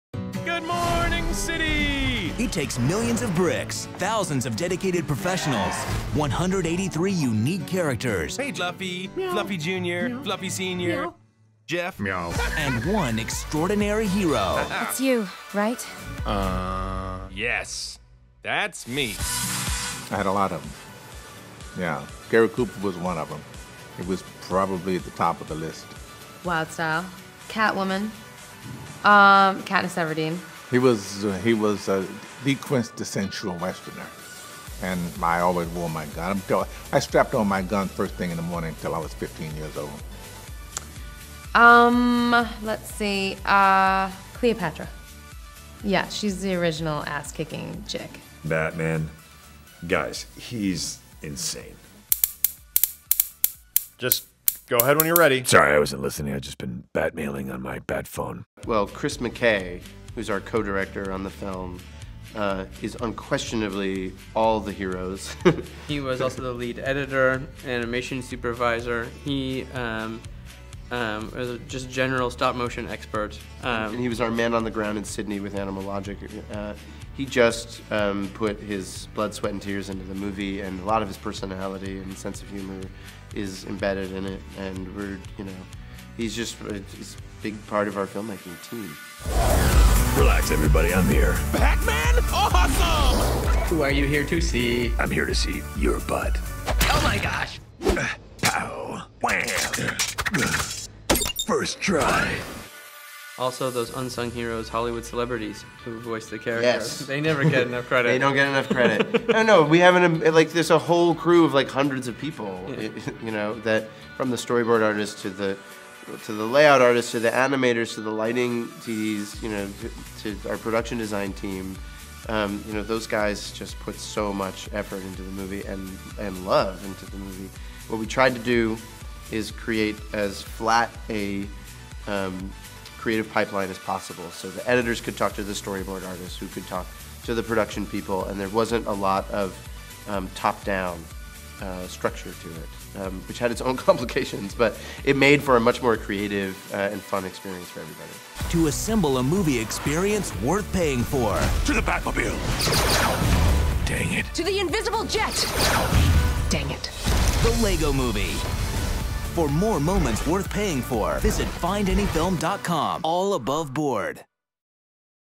LEGO exclusive interview with Morgan Freeman, Elizabeth Banks and Will Arnett
Who are your movie heroes?